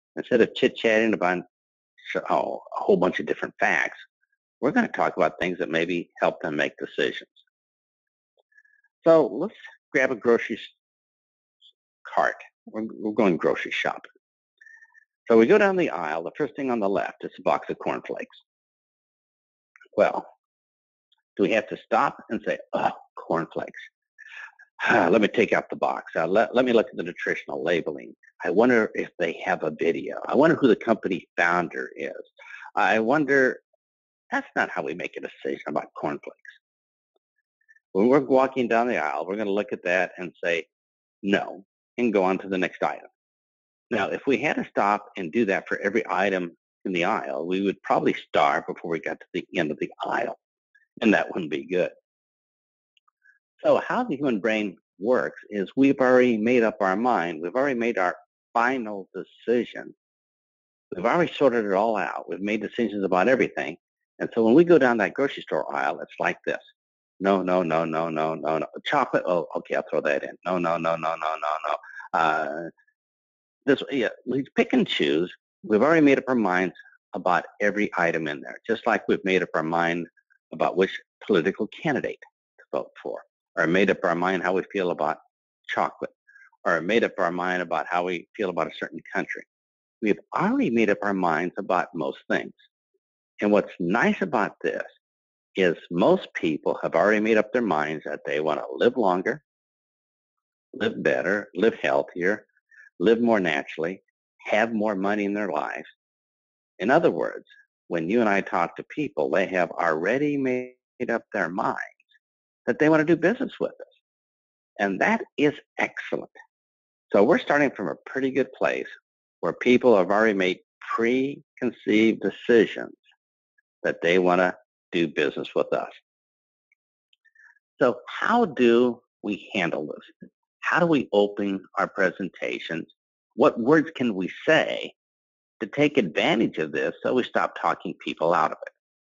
16.69 Tom (Big Al) Schreiter; How our Prospects Make Up their Mind Trailer
Tom (Big Al) Schreiter’s 2016 YL Success Summit presentation.

To view the entire presentation, register for the 48-hour FREE access pass here;